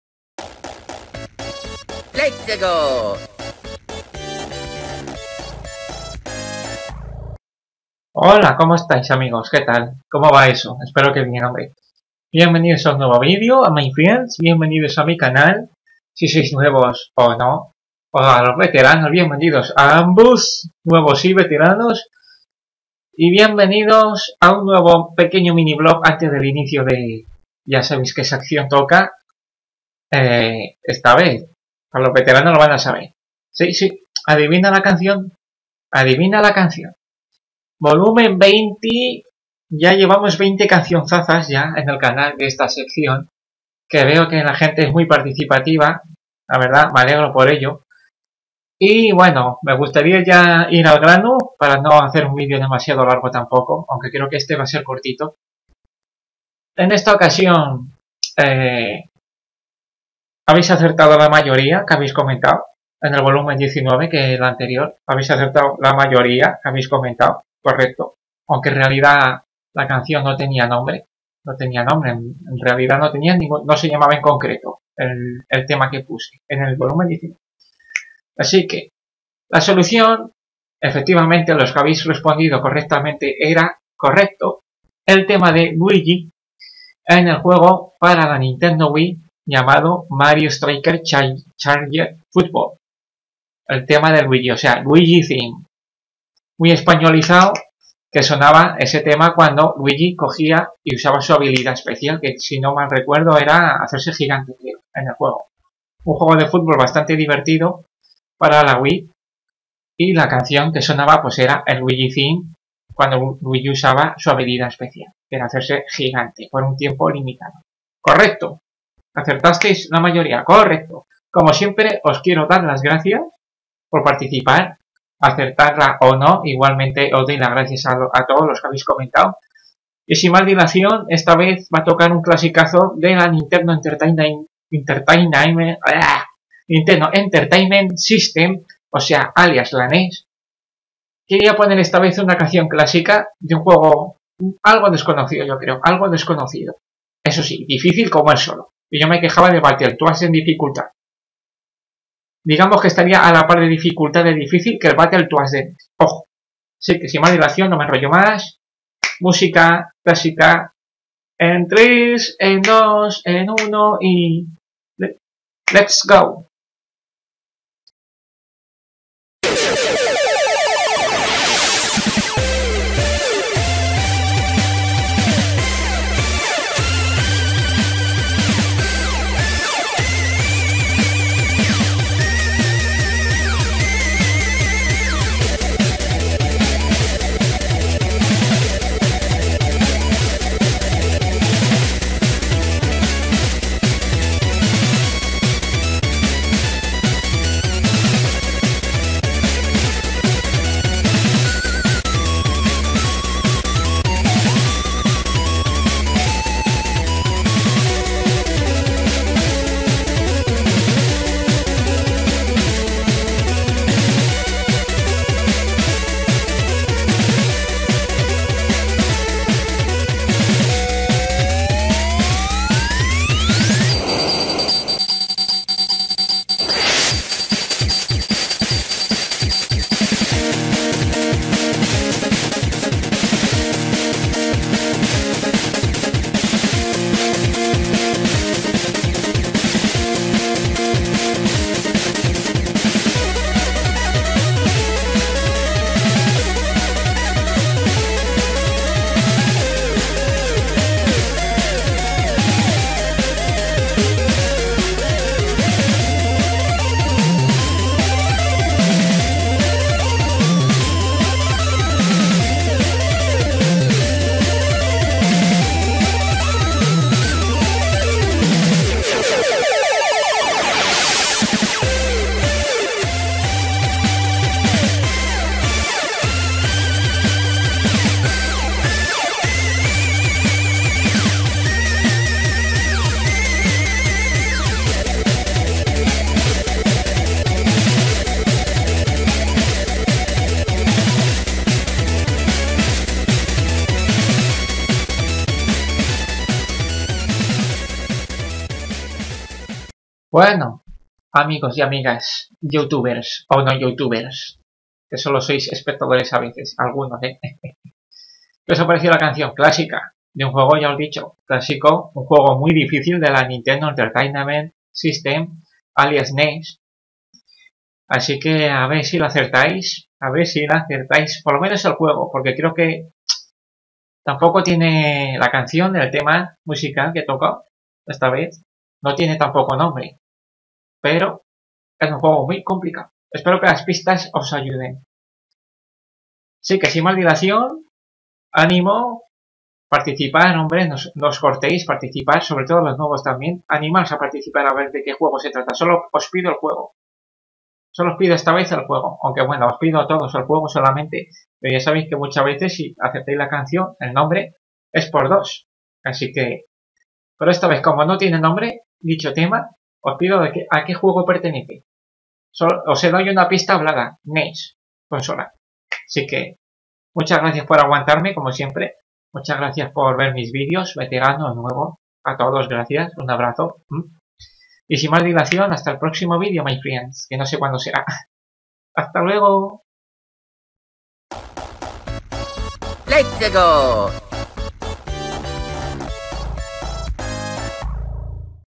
Adivinar la Canción ¿De que Canción y a que juego pertenece? Vol 20
Ya van 20 temas musicales para adivinar. ¿Sereis capaces de adivinarlo? C´mon.